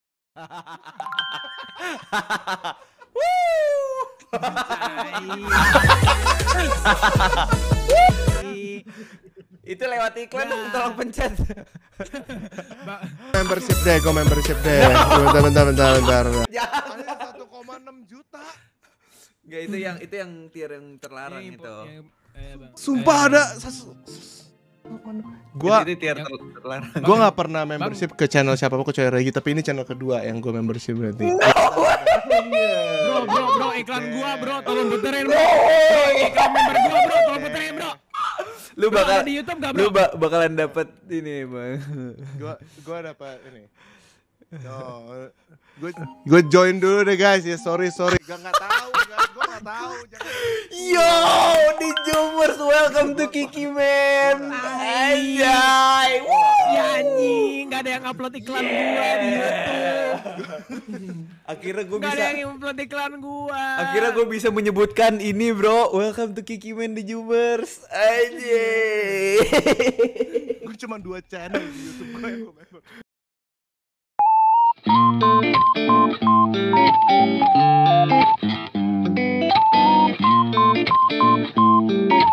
Momen bang Hans the jooomers join membership langsung di depan bang al, | naplive [clip]
[deskripsi]

pov bang al

ini adalah moment colab dadakan yang awalnya bang al andhika nug, akil dan om ray.. ketika akil out mereka mengajak strimer" grading SSS

dan ternyata the joomers aka bang hans mau di ajak join squad haruno gen 5 kwkwk

naplive
random mediashare
bang al
naplive clip